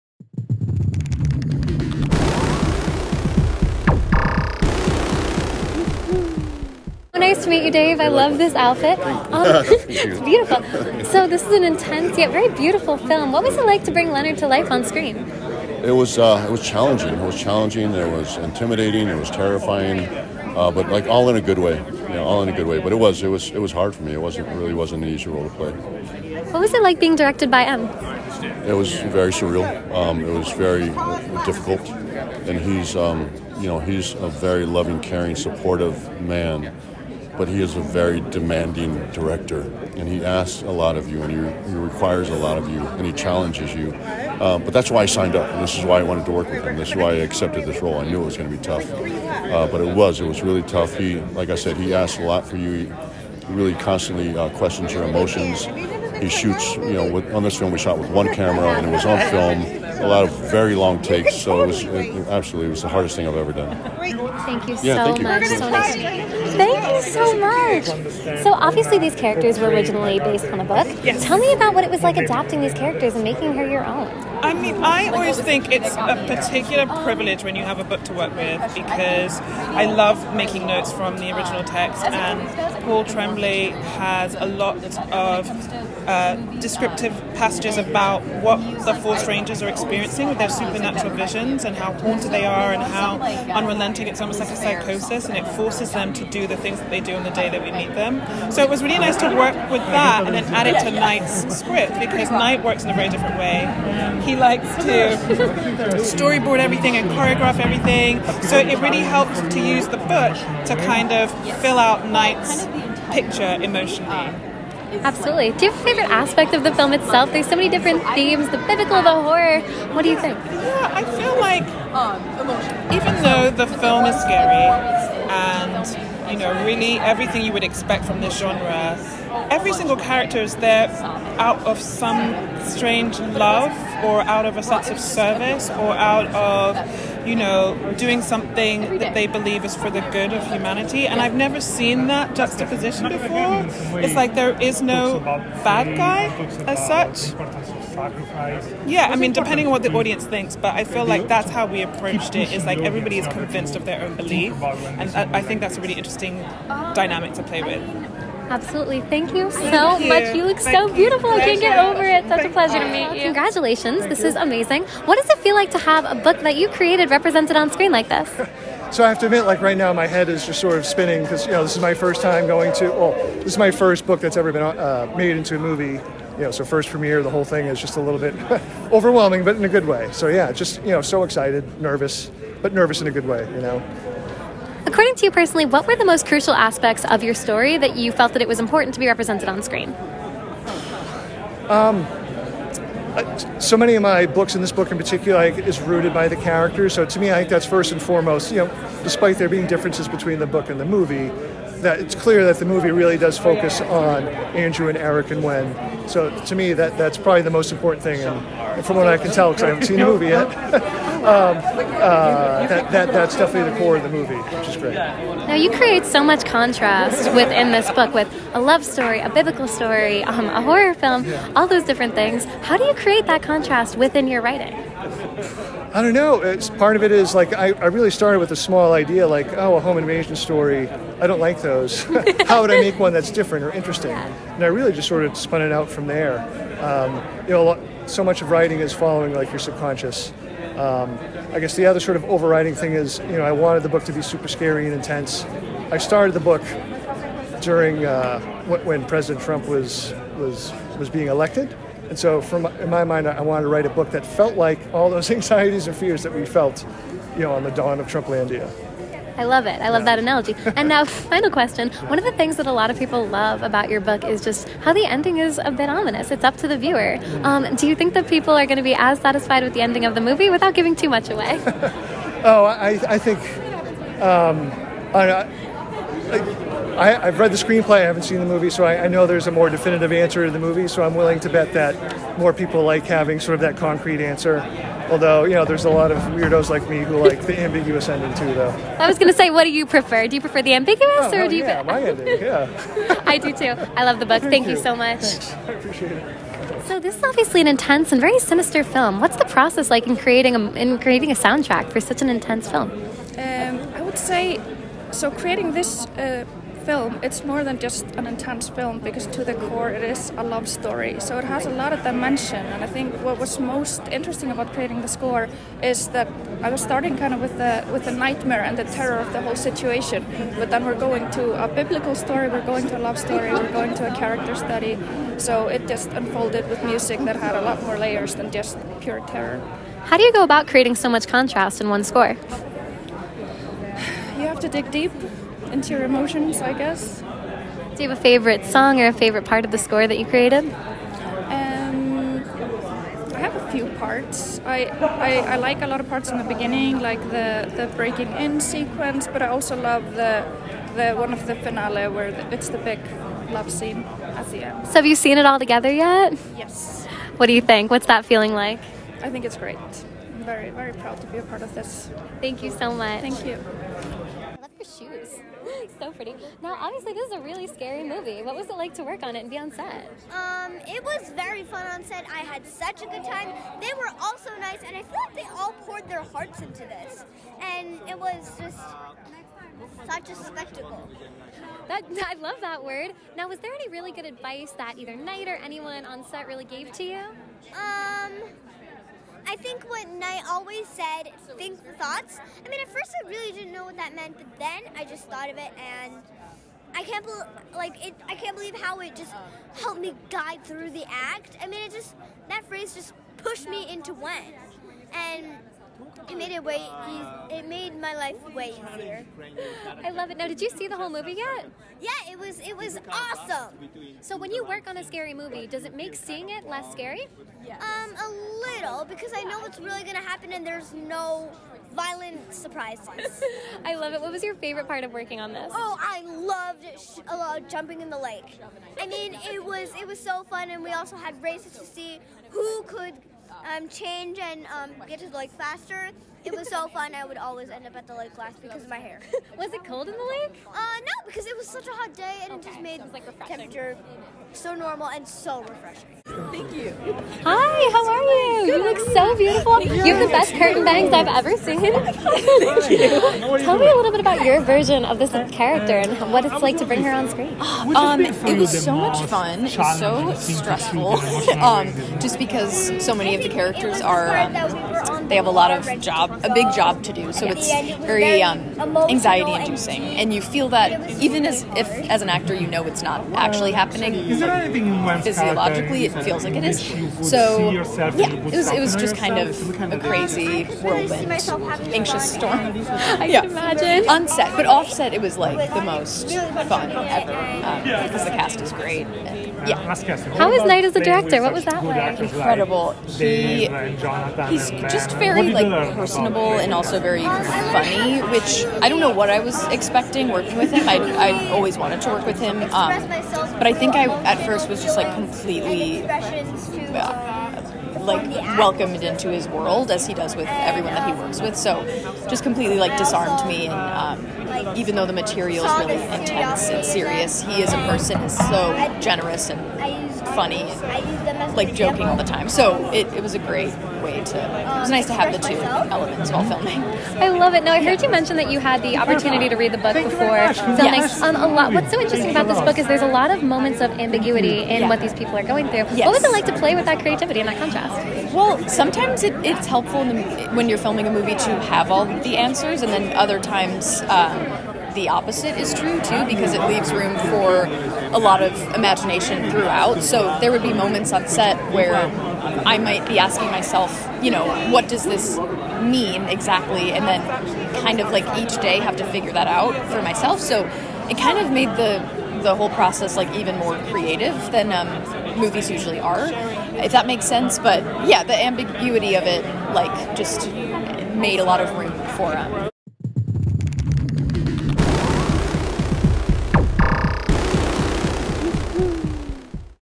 Dave Bautista, Nikki Amuka-Bird, Paul G. Tremblay, Kristen Cui & More Chat 'Knock at the Cabin'
While vacationing at a remote cabin in the woods, a young girl and her parents are taken hostage by four armed strangers who demand they make an unthinkable choice to avert the apocalypse. Confused, scared and with limited access to the outside world, the family must decide what they believe before all is lost.